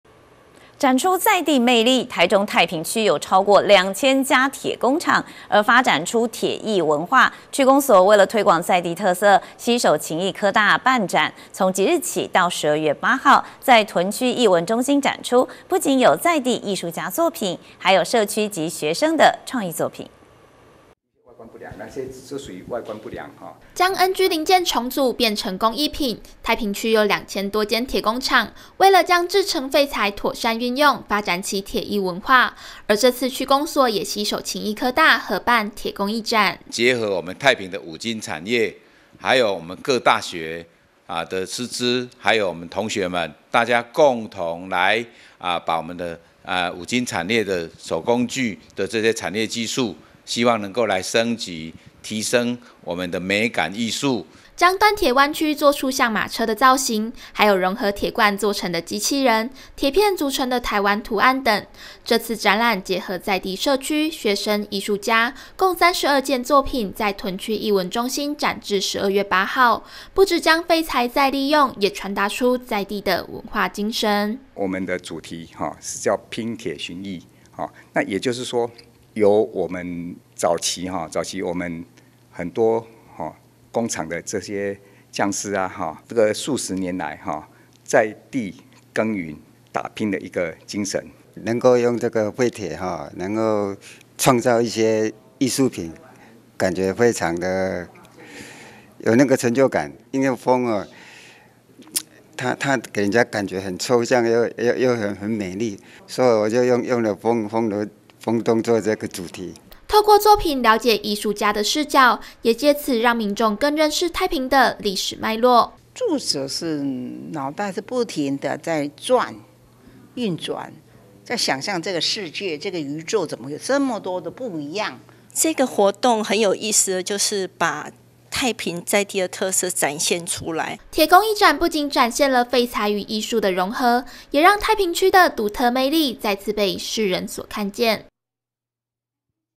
推廣在地鐵工藝術 公所攜手科大辦展│T-NEWS聯播網
展出在地魅力，台中太平區有超過2千家鐵工廠，而發展出鐵藝文化，區公所為了推廣在地特色，攜手勤益科大辦展，從即日起到12/8，在屯區藝文中心展出，不僅有在地藝術家作品，還有社區及學生的創意作品。